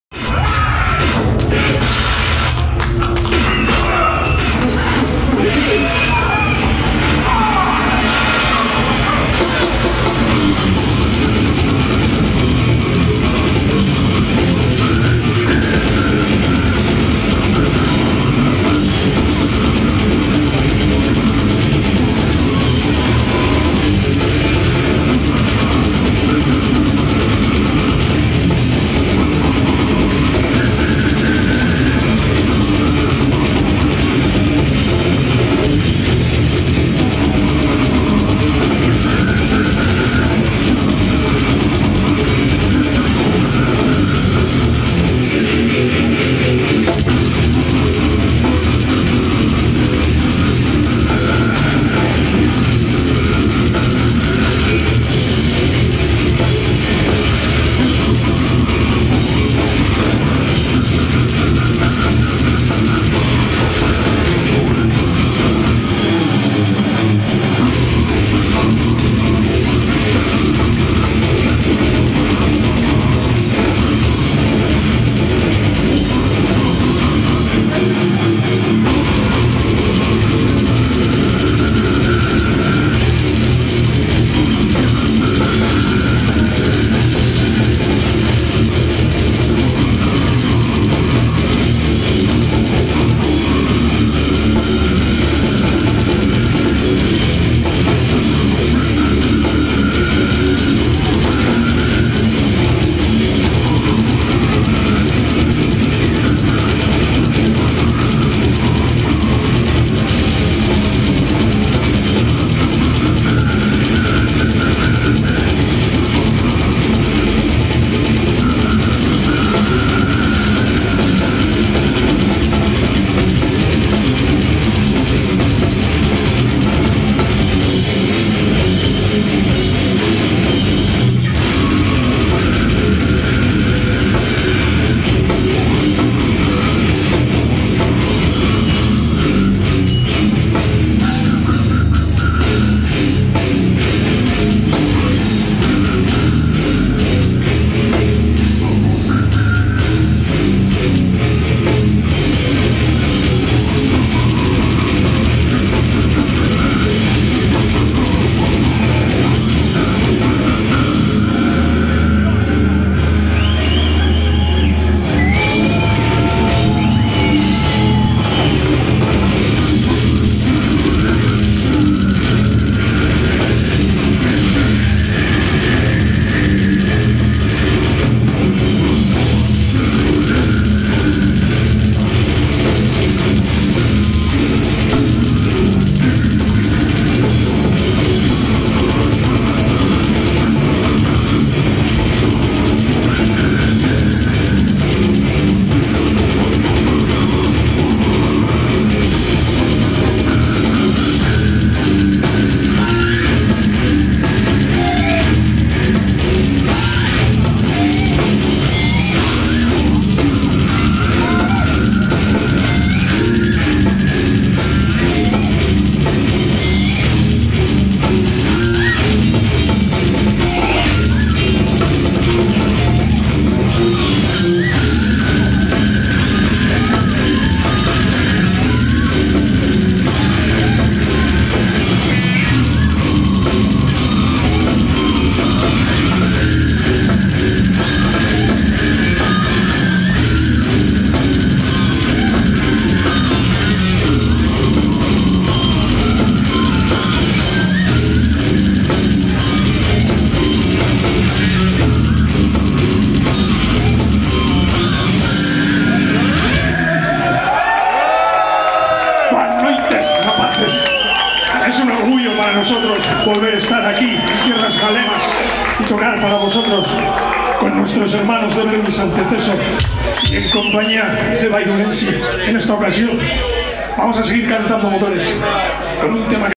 CEREBRAL EFFUSION - 2/9 (Brutal Northern Night, Sala Berlín 29/10/2016)
Ultra Brutal Basque band, CEREBRAL EFFUSION delivering pure brutality at "Brutal Northern Night" in Sala Berlín, Ourense.